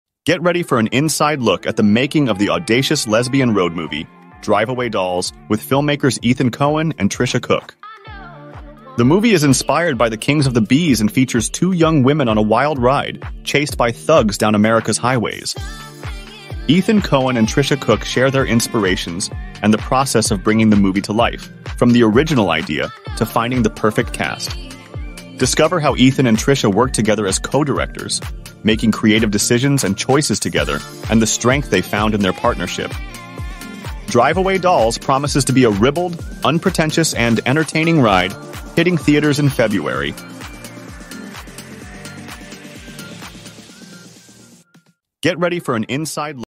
Ethan Coen and Tricia Cooke Answer Questions About Making Queer Caper ‘Drive-Away Dolls’
► Title: - Ethan Coen and Tricia Cooke Answer All Our Impertinent Questions About Making Queer Caper ‘Drive-Away Dolls’